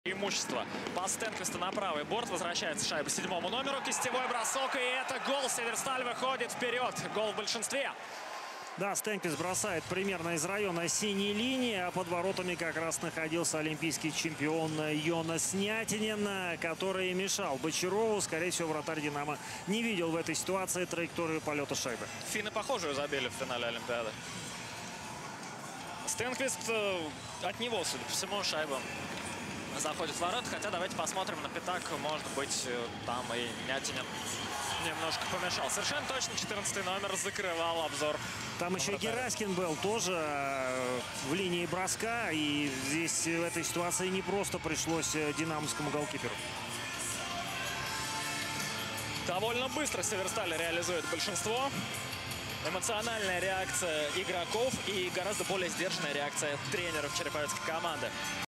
Stenqvist gives SEV lead
Stenqvist fires one to give SEV lead in Gm1

Стенквист метко стреляет от синей